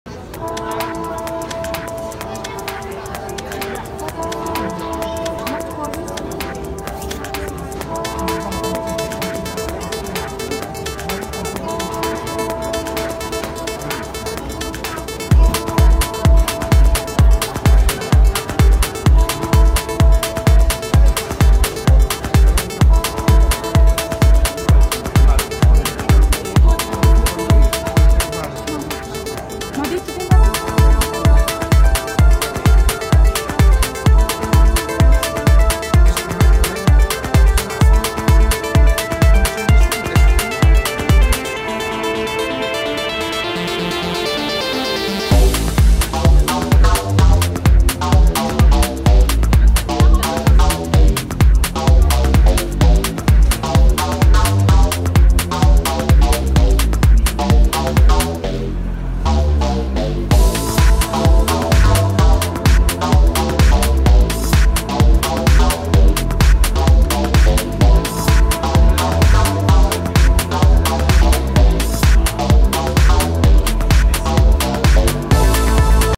Quick Car Review: 2017 Tesla Model X 90D
In this video I am going to review the 2017 Tesla Model X 90D.
It is a pretty quick review because there were so many people standing around the car.
Luckily I could film almost everything except some parts of the interior.